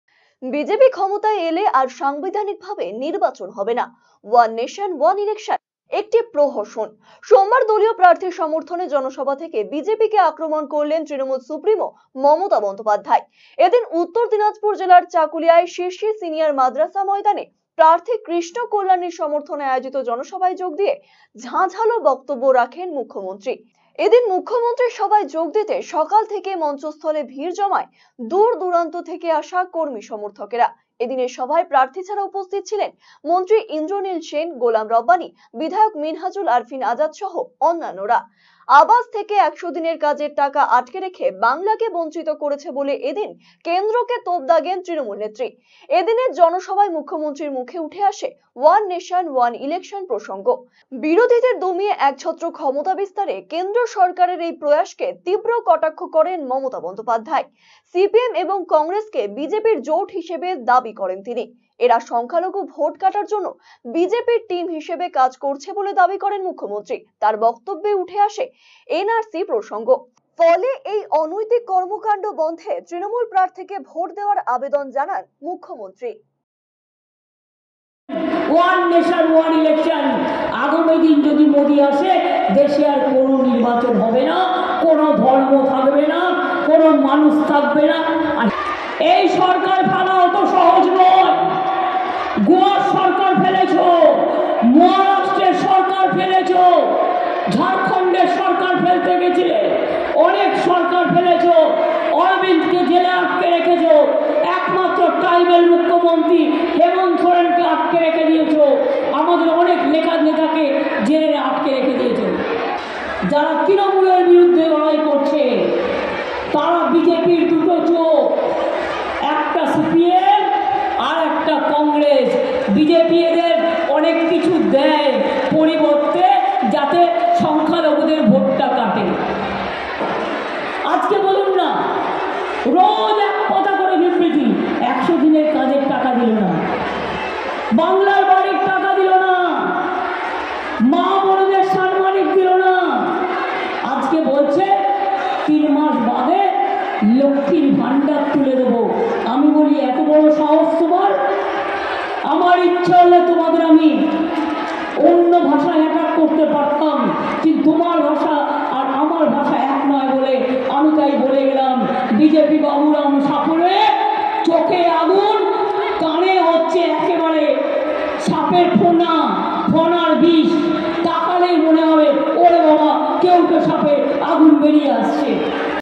Elections 2024 So Far | চাকুলিয়ায় মুখ্যমন্ত্রী | Rctv Sangbad
সংক্ষেপে খবরের বিষয়ঃ  বিজেপি ক্ষমতা এলে আর সাংবিধানিকভাবে নির্বাচন হবে না। ওয়ান নেশন ওয়ান ইলেকশন একটি প্রহসন। সোমবার দলীয় প্রার্থীর সমর্থনে জনসভা থেকে বিজেপিকে আক্রমণ করলেন তৃণমূল সুপ্রিমো মমতা বন্দ্যোপাধ্যায়৷